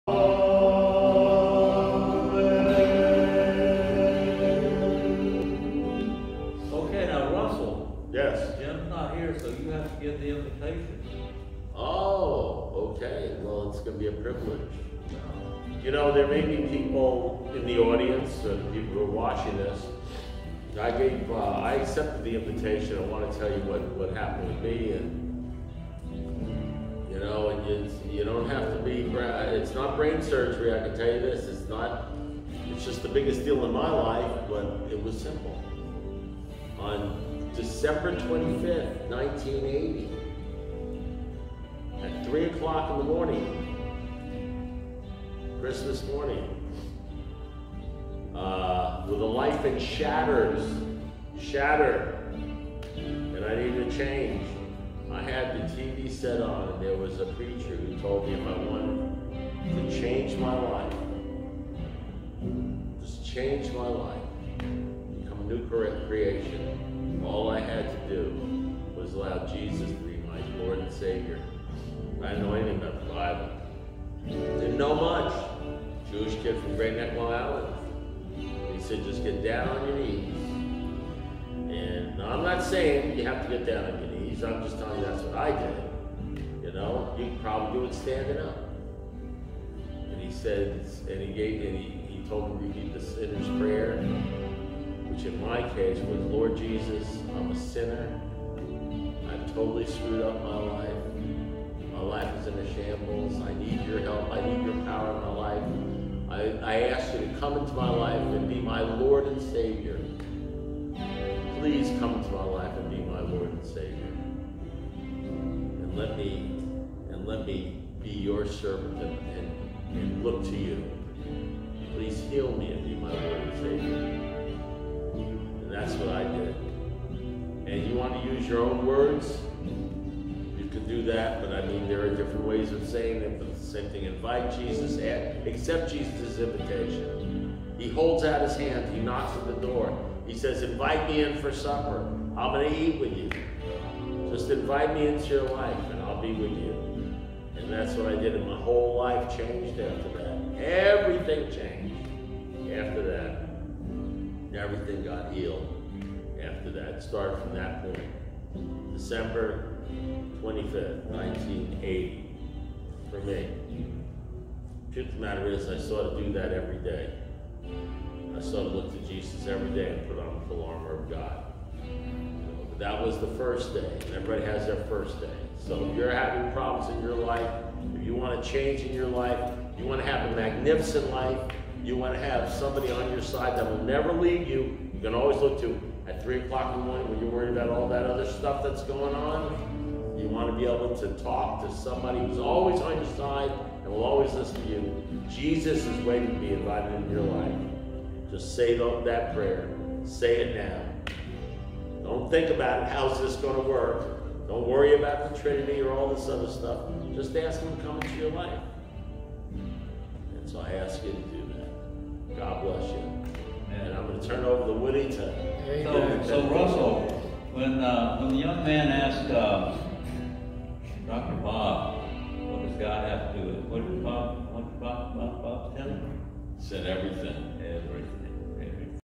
RUSSELL SPATZ *** TESTIMONY & INVITATION *** ALIVE AGAIN SUNDAY WORSHIP SERVICE *** DECEMBER 5, 2021
ALIVE AGAIN MINISTRIES
Pastor/Co-Founder: Dr. Michael C. "Woody" Woodham
Hymn Leader/Co-Founder: Russell Spatz
Prayer & Invitation Leader/Missionary: James Myrick
Bible Verses: Claude Perpignand
Vocalist/Guitar: Michael Vile
Vocalist: Dawn Moodie
Vocalist: John Arroyo

PRAYERS FOR PAULINE
Pianist: Pauline Russell (HYMN LYRICS BELOW)

Checks payable to AliveAgain
For either Woodham’s, Myrick’s, or Reid’s support

Mail to:
Attn: Dr. Michael Woodham